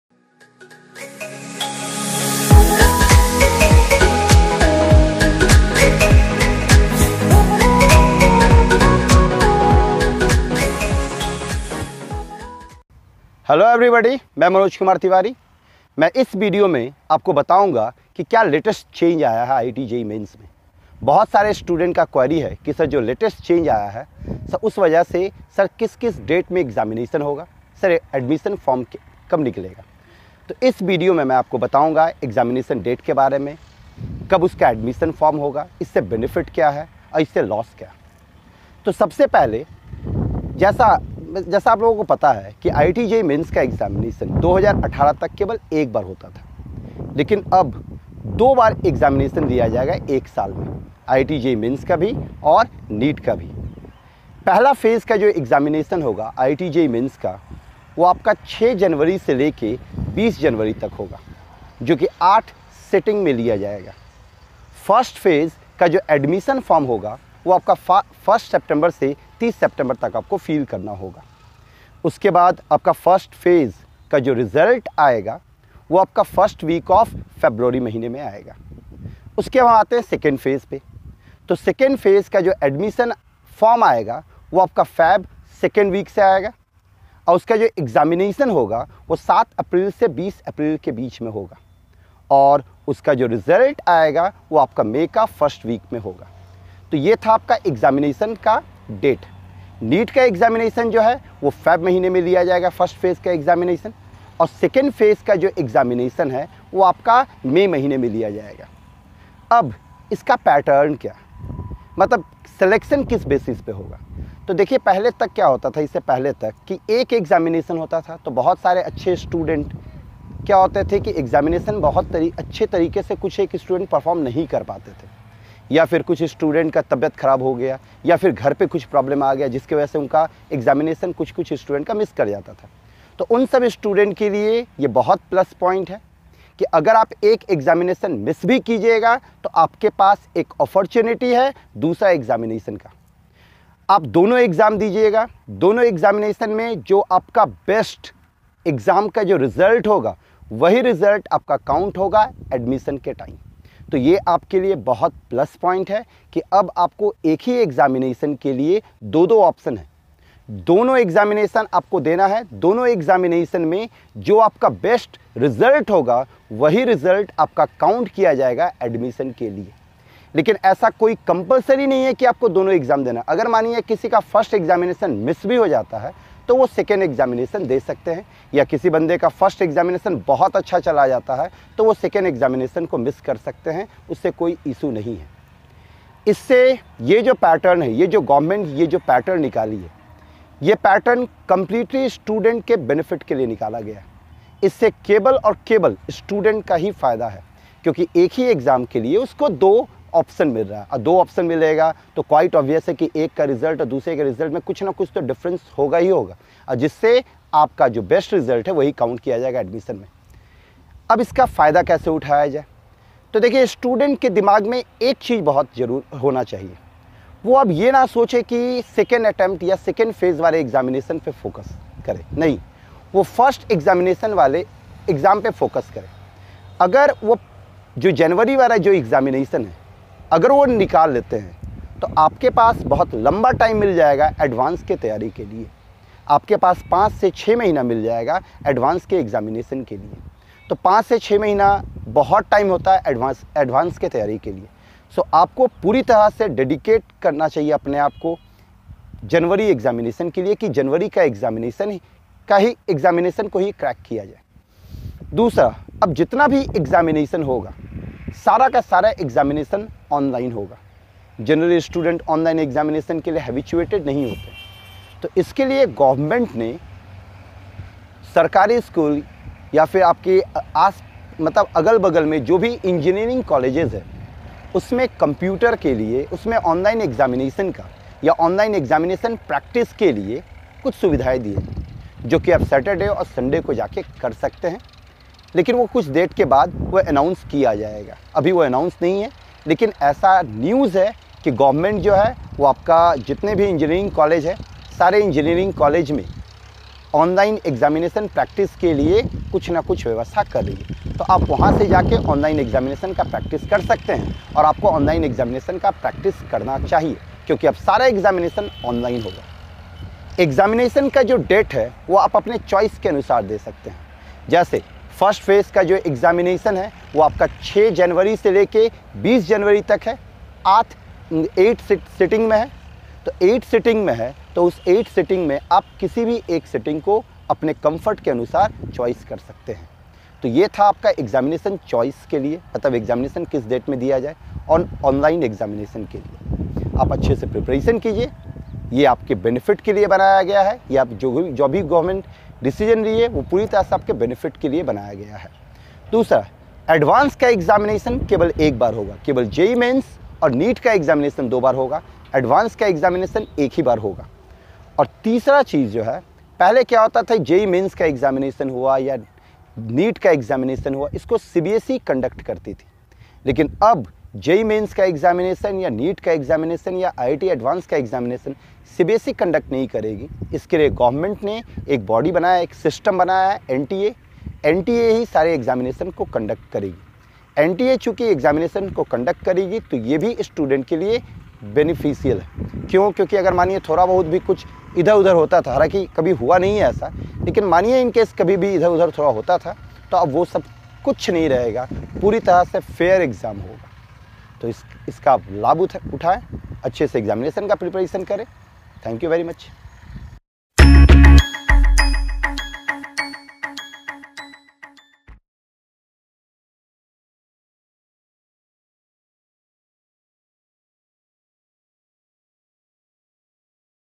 Important information on IITJEE MAINS 2019 | Manoj tiwary
Hello everyone! Here I am with another video for all the students aspiring for IIT JEE. This video is about the latest change made in the JEE main examination by CBSE. This video includes all the important information regarding this new pattern including the date of registration, date of examination, result declaration and other important details. It is a must watch video for all the JEE MAIN aspirants.